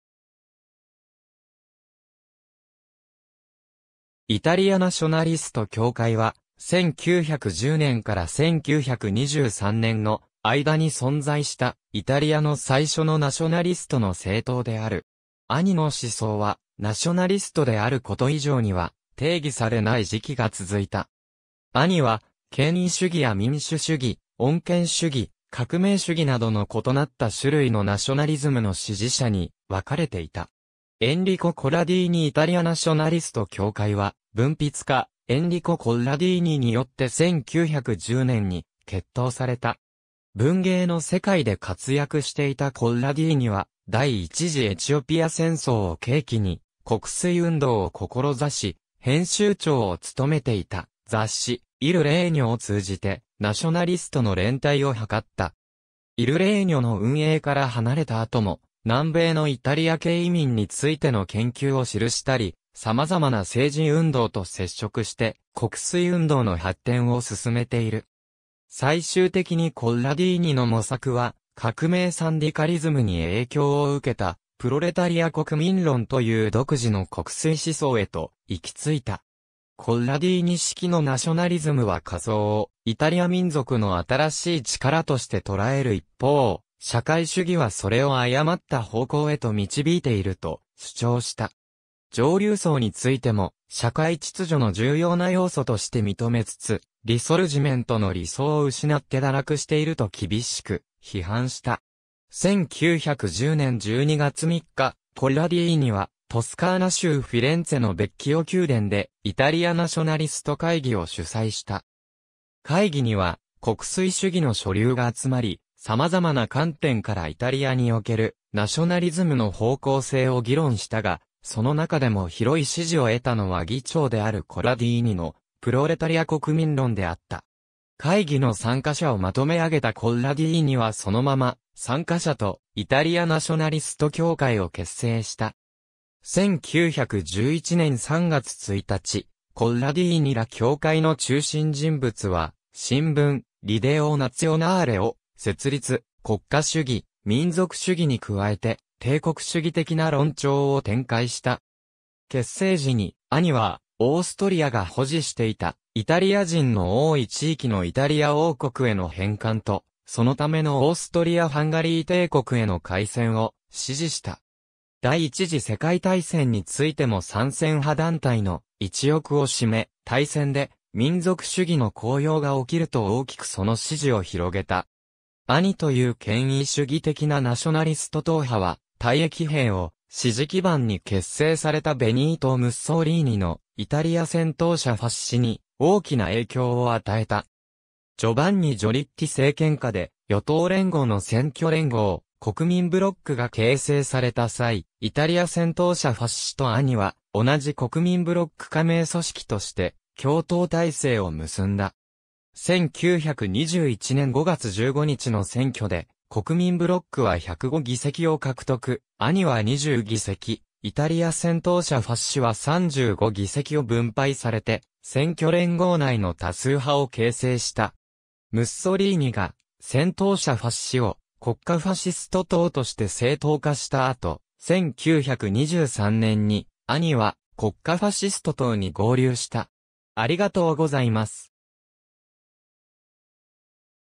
イタリア・ナショナリスト協会
イタリア・ナショナリスト協会（イタリア・ナショナリストきょうかい、イタリア語: Associazione Nazionalista Italiana、英語: Italian Nationalist Association、ANI）は、1910年から1923年の間に存在したイタリアの最初のナショナリストの政党である。
ANIの思想は、ナショナリストであること以上には、定義されない時期が続いた。
ANIは、権威主義や民主主義、穏健主義、革命主義などの異なった種類のナショナリズムの支持者に分かれていた。
エンリコ・コッラディーニ イタリア・ナショナリスト協会（ANI）は文筆家エンリコ・コッラディーニによって1910年に結党された。
文芸の世界で活躍していたコッラディーニは第一次エチオピア戦争を契機に国粋運動を志し、編集長を務めていた雑誌『イル・レーニョ』を通じてナショナリストの連帯を図った。
『イル・レーニョ』の運営から離れた後も南米のイタリア系移民についての研究を記したり、様々な政治運動と接触して国粋運動の発展を推めている。
最終的にコッラディーニの模索は革命サンディカリズムに影響を受けた「プロレタリア国民論」という独自の国粋思想へと行き着いた。
コッラディーニ式のナショナリズムは下層をイタリア民族の新しい力として捉える一方、社会主義はそれを誤った方向へと導いていると主張した。
上流層についても社会秩序の重要な要素として認めつつ、リソルジメントの理想を失って堕落していると厳しく批判した。
1910年12月3日、コッラディーニはトスカーナ州フィレンツェのヴェッキオ宮殿でイタリア・ナショナリスト会議を主催した。
会議には国粋主義の諸流が集まり、様々な観点からイタリアにおけるナショナリズムの方向性を議論したが、その中でも広い支持を得たのは議長であるコッラディーニの「プロレタリア国民論」であった。
会議の参加者を纏め上げたコッラディーニはそのまま参加者とイタリア・ナショナリスト協会を結成した。
1911年3月1日、コッラディーニら協会の中心人物は新聞『リデオ・ナツィオナーレ』を設立、国家主義・民族主義に加えて帝国主義的な論調を展開した。
結成時にANIはオーストリアが保持していたイタリア人の多い地域のイタリア王国への返還と、そのためのオーストリア＝ハンガリー帝国への開戦を支持した。
第一次世界大戦についても参戦派団体の一翼を占め、大戦で民族主義の高揚が起きると大きくその支持を広げた。
ANIという権威主義的なナショナリスト党派は、退役兵を支持基盤に結成されたベニート・ムッソリーニのイタリア戦闘者ファッシ（イタリア語: Fasci italiani di combattimento）に大きな影響を与えた。
ジョヴァンニ・ジョリッティ政権下で与党連合の選挙連合「国民ブロック」が形成された際、イタリア戦闘者ファッシとANIは同じ国民ブロック加盟組織として共闘体制を結んだ。
1921年5月15日の選挙で国民ブロックは105議席を獲得、ANIは20議席、イタリア戦闘者ファッシは35議席を分配されて選挙連合内の多数派を形成した。
ムッソリーニが戦闘者ファッシを国家ファシスト党（PNF）として政党化した後、1923年にANIは国家ファシスト党に合流した。